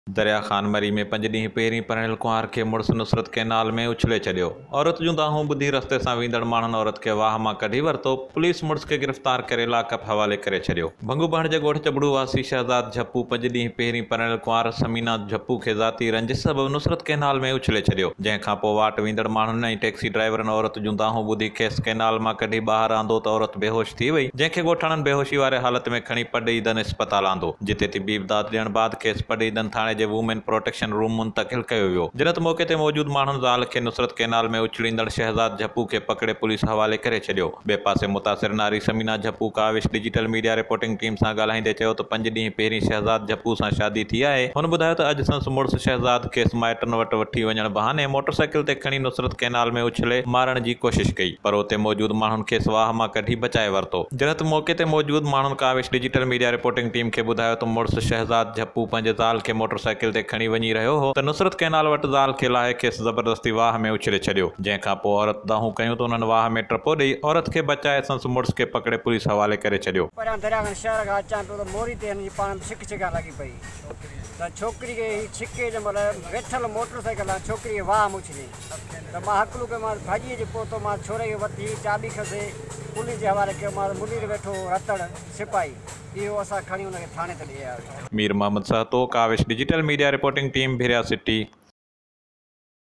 Daily Kawish News Update | 04.07.2019 | 5:00 PM | Thursday
ڀرياسٽي: 5 ڏينهن اڳ پرڻيل ڪنوار کي مڙس واھ ۾ اڇلي ڇڏيو، عورت جي دانهن تي ماڻهن بچائي ورتو